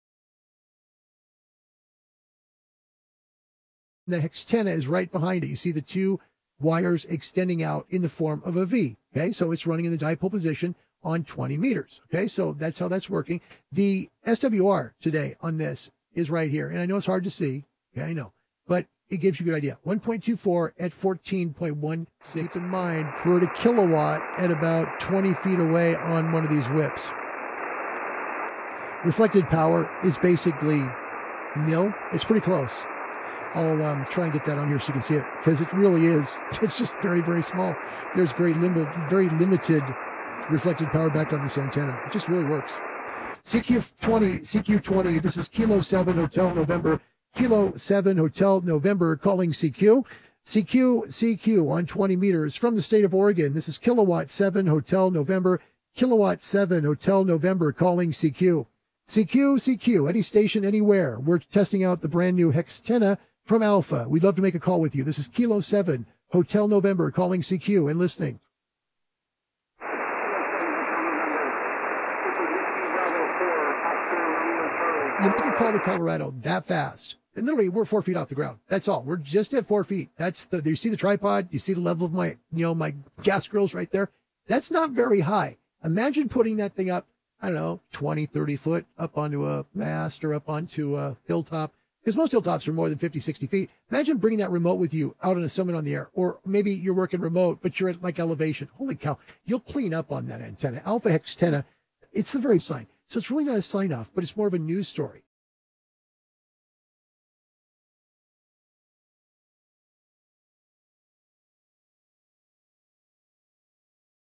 HexTenna QSO - SUPER LOW SWR at 1KW
Here is a the HexTenna handling 1KW showing an extremely low SWR and nearly zero reflected power.